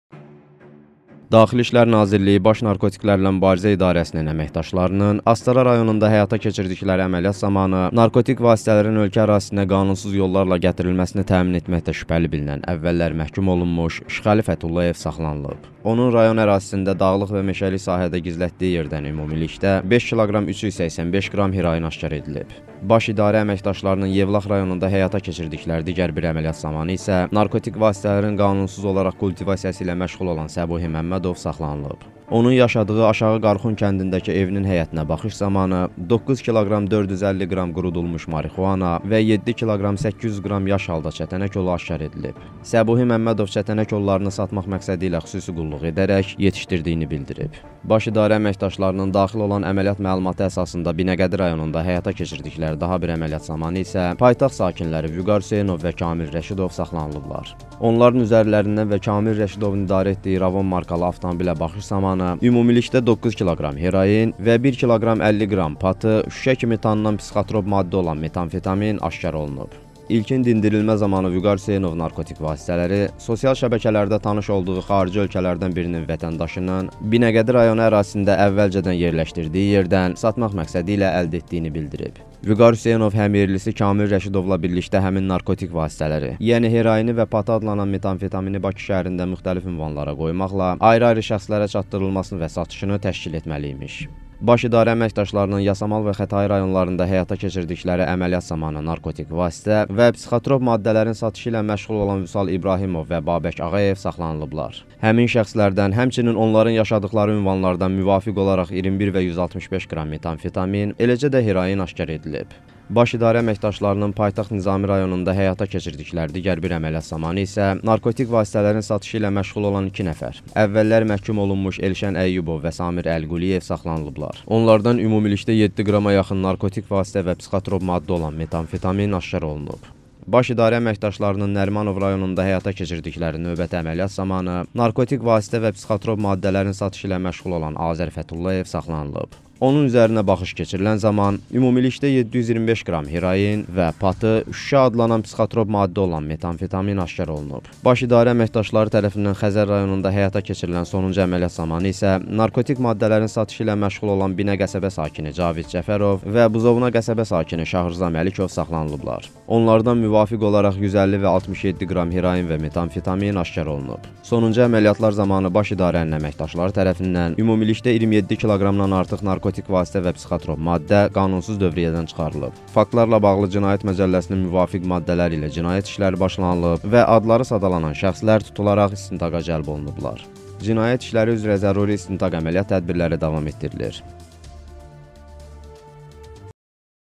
Narkotacirlərə qarşı əməliyyat
Bu dəfə əməliyyat vətəndaşların göndərdiyi məlumatlar əsasında baş tutub